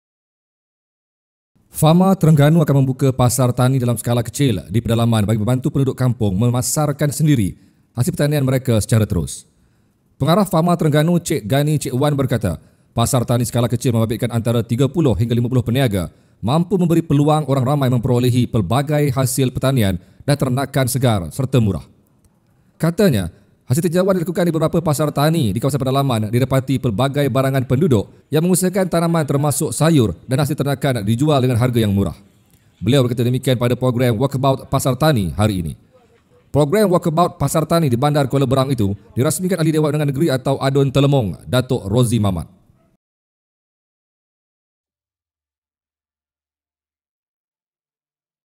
Pasar tani skala kecil di pedalaman
KUALA BERANG: FAMA Terengganu akan membuka pasar tani dalam skala kecil di pedalaman bagi membantu penduduk kampung memasarkan sendiri hasil pertanian secara terus.